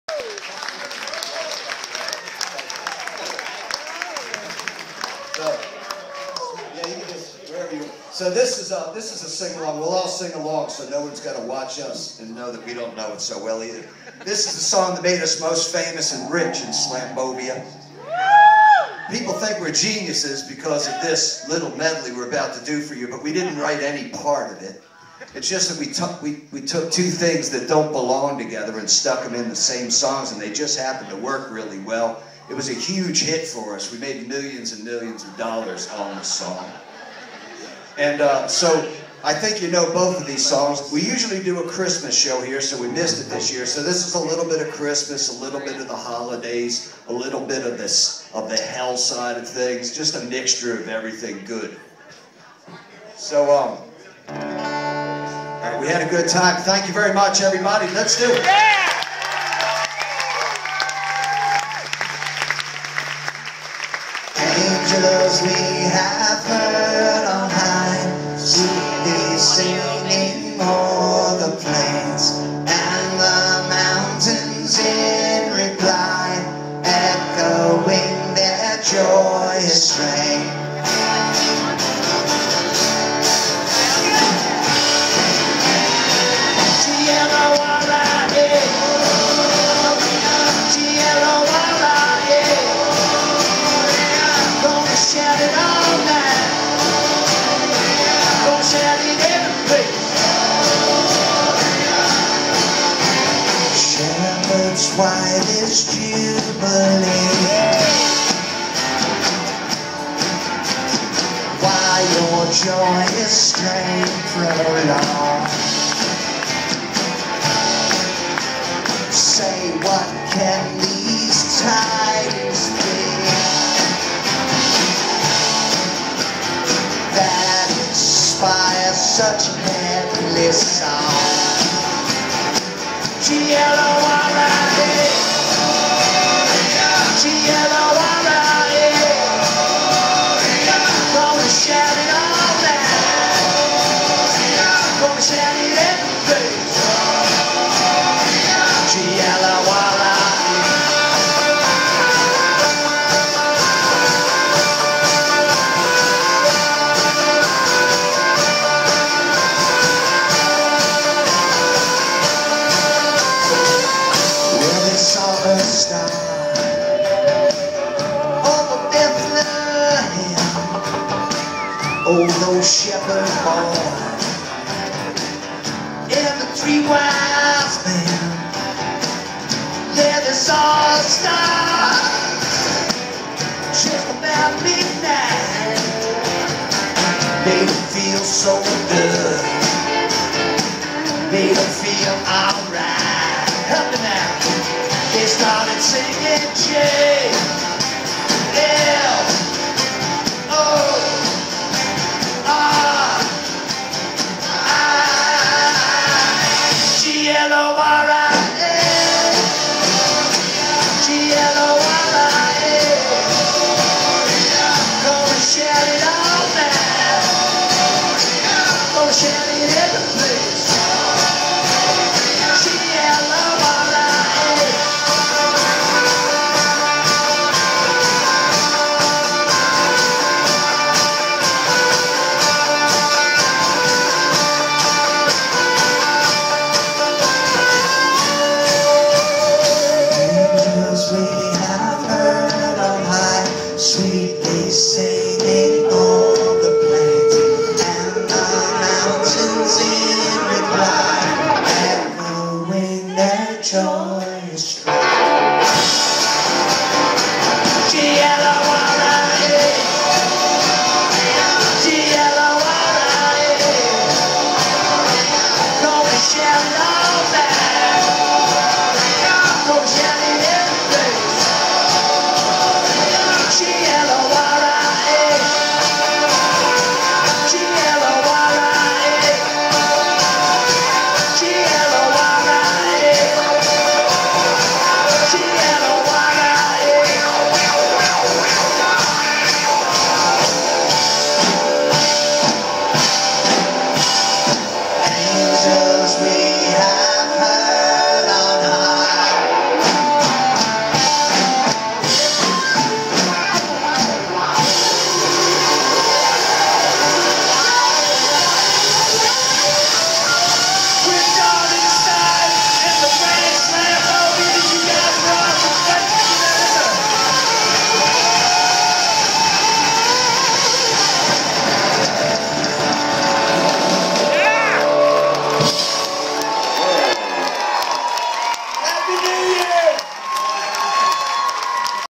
Darlingside with the grand slambovians
The Grand Slambovians invite Darlingside to sing along in their encore at their 1/12 show at Infinity Hall